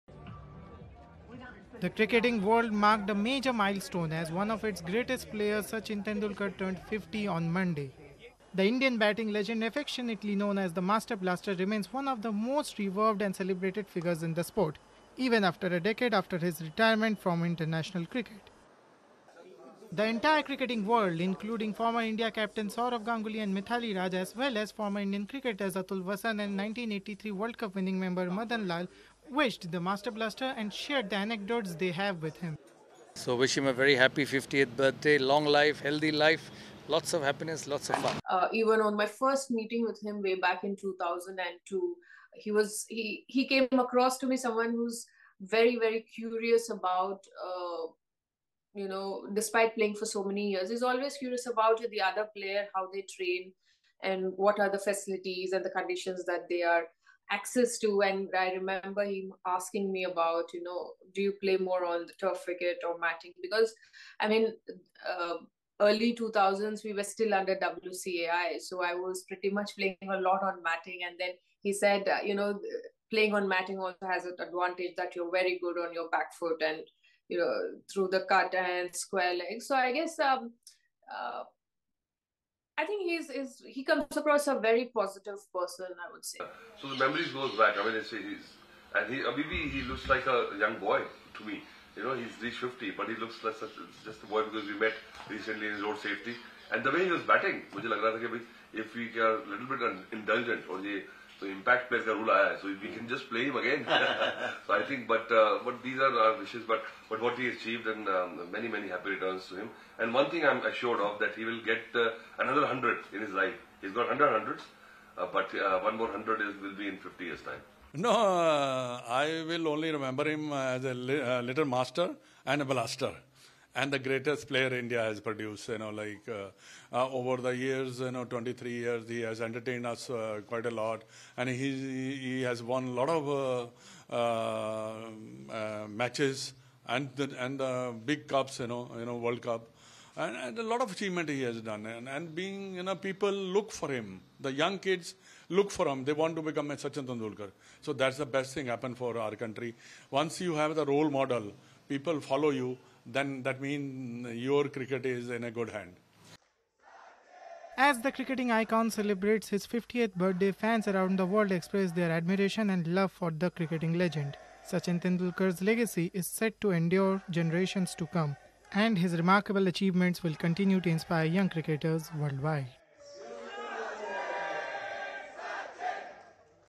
Sachin Tendulkar, 50th B'day: Legends Share Master Blaster's Unheard Tales
The cricketing world marked a major milestone as one of its greatest players, Sachin Tendulkar, turned 50 years old on Monday. The Indian batting legend, affectionately known as the Master Blaster, remains one of the most revered and celebrated figures in the sport, even a decade after his retirement from international cricket. The entire cricketing world, including former India captains Sourav Ganguly and Mithali Raj, as well as former Indian cricketers Atul Wassan and 1983 World Cup winning member Madan Lal, wished the Master Blaster and shared anecdotes they have with him. (Video credit: PTI)